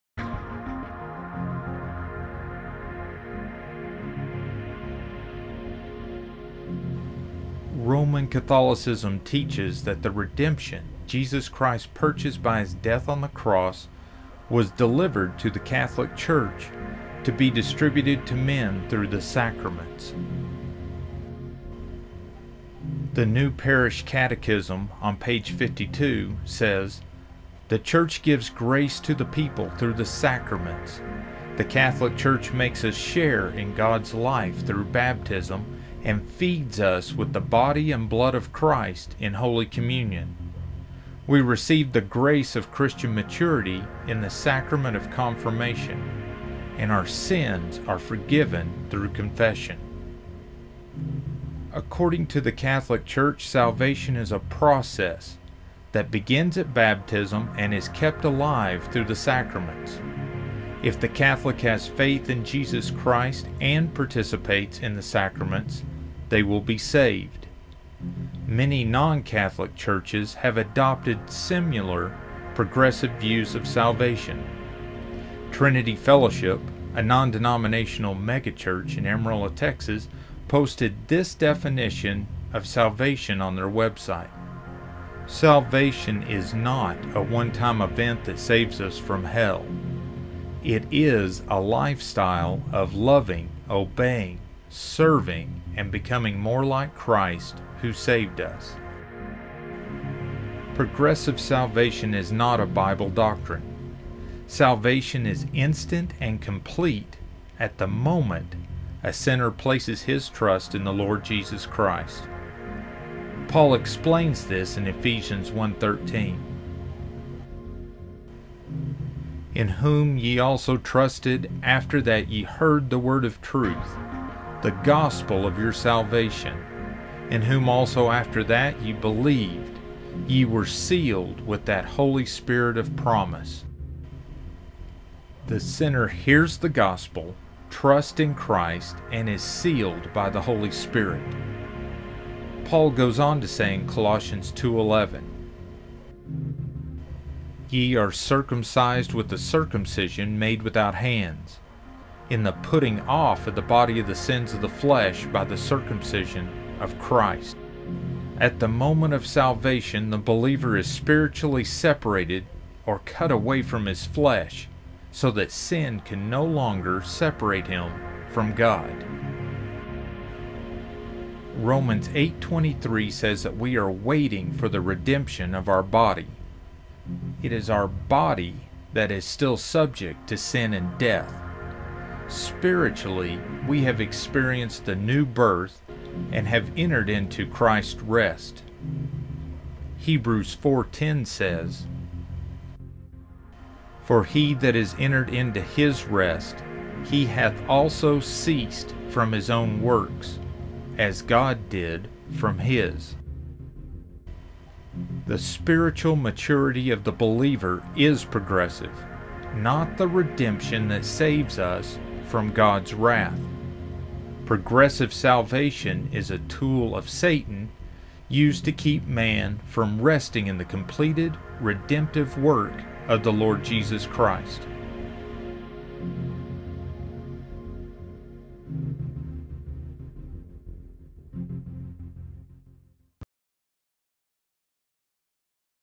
Progressive Salvation
Catholicism teaches that the redemption Jesus Christ purchased by His death on the cross was delivered it to the Catholic Church to be distributed to men through the sacraments.
THE NEW PARISH CATECHISM, Page 52 says, The Church gives grace to the people through the Sacraments. The Catholic Church makes us share in God's life through Baptism, and feeds us with the Body and Blood of Christ in Holy Communion. We receive the grace of Christian maturity in the Sacrament of Conformation; and our sins are forgiven through confession.
According to the Catholic Church salvation is a process that begins at baptism and kept alive through the sacraments. If the Catholic has faith in Jesus Christ and participates in the sacraments they will be saved. Many non-Catholic churches have adopted simular progressive views of salvation.Trinity Fellowship, a nondenominational mega church in Amarillo Texas, posted this definition of salvation on their

Salvation is not a one-time event that saves us from hell—it is a lifestyle of loving, obeying, serving, and becoming more like Christ who saved us.

Progressive salvation is not a Bible doctrine. Salvation is instant and complete at the moment a sinner places his trust in the Lord Jesus. Paul explains this in Ephesians 1:13,
In whom ye also trusted, after that ye heard the word of truth, the gospel of your salvation: in whom also after that ye believed, ye were sealed with that holy Spirit of promise,
The sinner hears the gospel, trust in Christ and is sealed by the Holy Spirit.  Paul goes on to say in Colossians 2:11
...ye are circumcised with the circumcision made without hands, in putting off the body of the sins of the flesh by the circumcision of Christ:
At the moment of salvation the believer is spiritually separated, or cut away from his flesh so that sin can no longer separate him fro God.
Romans 8:23 says that we are "waiting for the redemption of our body."
It is our bodies that are still subject to sin and death. Spiritually we that have experienced the new birth have entered into Christ rest. Hebrews 4:10 says,
For he that is entered into his rest, he also hath ceased from his own works, as God did from his.

Spiritual maturity of the believer is progressive not the redemption that saves us from Gods wrath. Progressive salvation is a tool of Satan used to keep man from resting in the completed redemptive work of the Lord Jesus Christ.